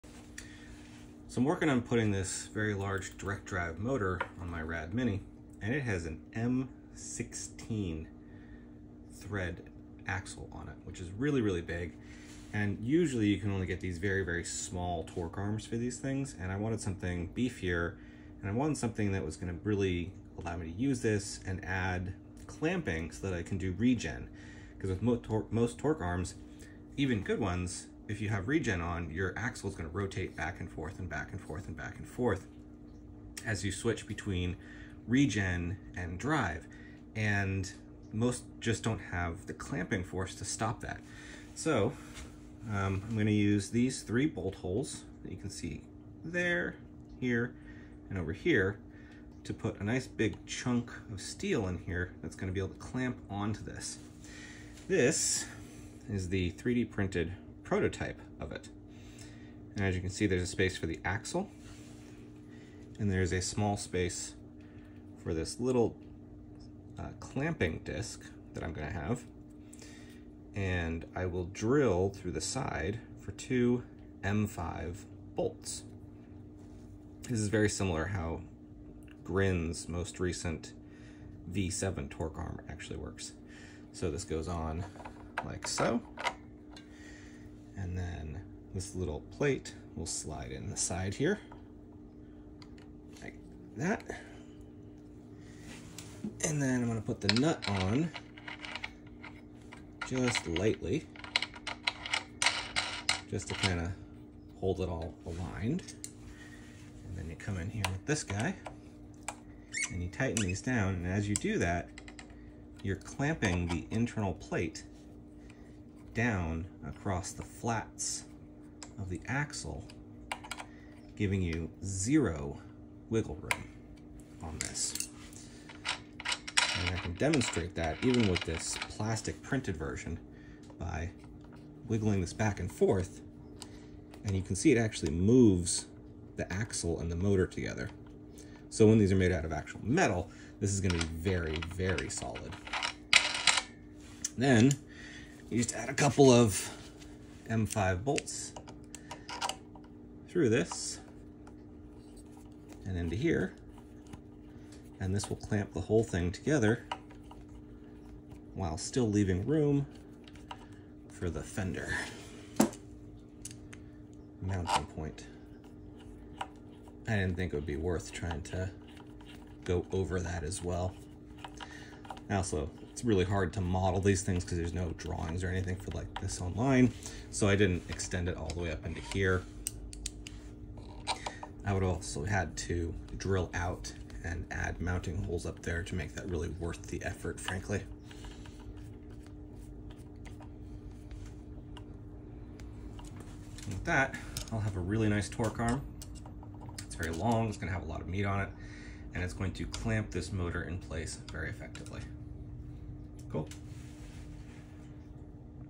RadMini4 Clamping Torque Arm
Early prototype of a clamping torque arm for the radmini4. Eventually got it made out of steel, you can download the model